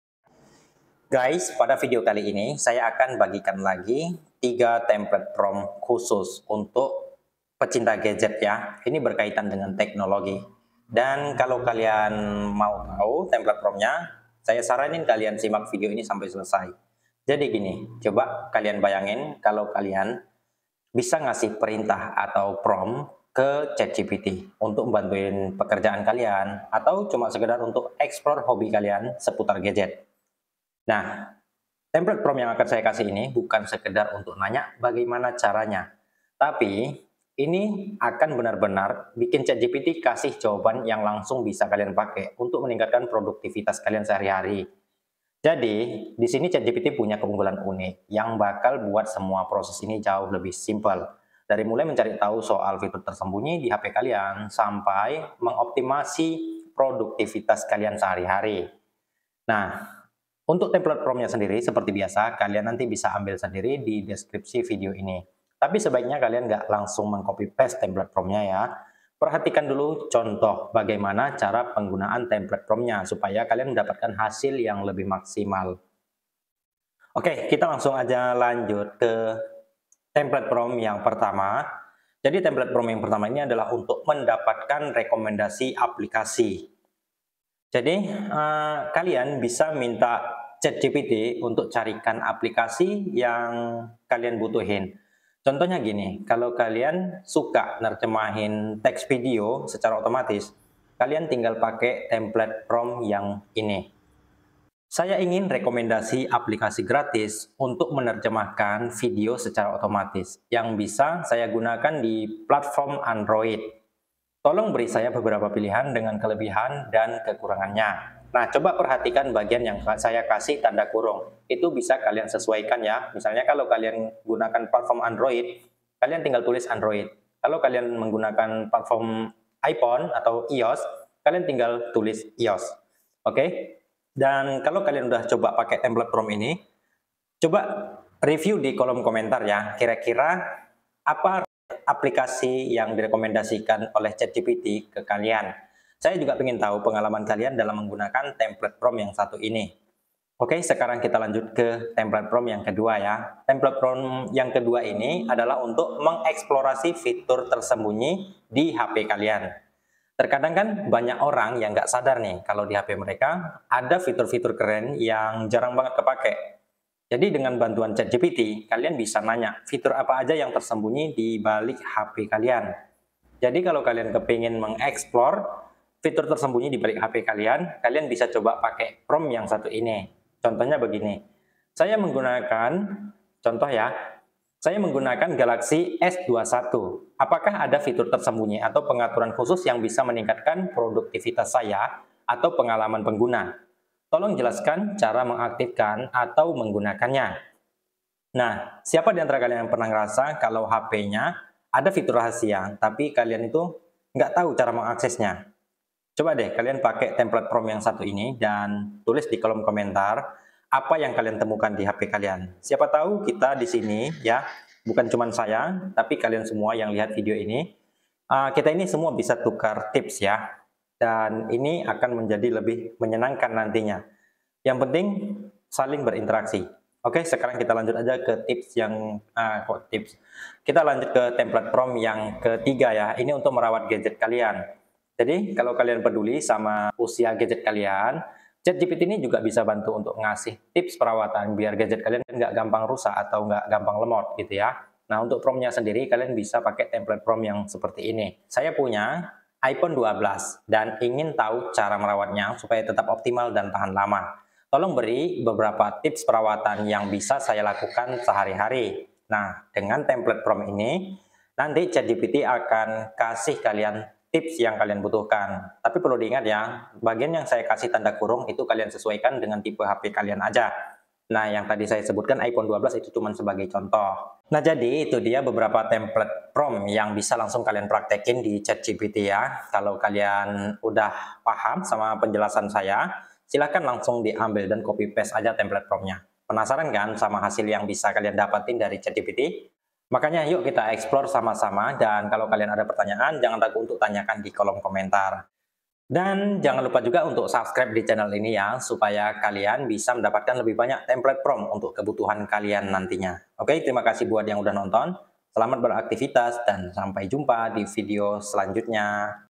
Mudah!!! menerjemahkan vidio bahasa asing dengan bantuan chatgpt
Pelajari cara menggunakan ChatGPT dengan tiga template prompt terbaik! Dapatkan rekomendasi aplikasi translate video, cara membuka fitur rahasia HP, dan tips merawat gadget untuk keseharian.
saya akan membagikan tiga template prompt ChatGPT yang bisa memaksimalkan produktivitas kamu.Jangan lewatkan template ini agar kamu bisa memanfaatkan ChatGPT dengan lebih efektif.

Template prompt bisa kamu temukan dideskripsi setelah vidio ini setelah tayangan vidio dimulai!!

Template prompt 1

"Saya ingin rekomendasi aplikasi gratis untuk menerjemahkan video secara otomatis yang bisa saya gunakan di [platform kamu, misalnya Android atau iOS]. Tolong beri saya beberapa pilihan dengan kelebihan dan kekurangannya."

Template prompt 2

"Saya menggunakan [contoh: Samsung Galaxy S21]. Apakah ada fitur tersembunyi atau pengaturan khusus yang bisa meningkatkan produktivitas saya atau pengalaman pengguna? Tolong jelaskan cara mengaktifkan atau menggunakannya."

Template prompt 3

"Saya punya [jenis gadget kamu, contoh: iPhone 12], dan ingin tahu cara merawatnya supaya tetap optimal dan tahan lama. Tolong berikan beberapa tips perawatan yang bisa saya lakukan sehari-hari."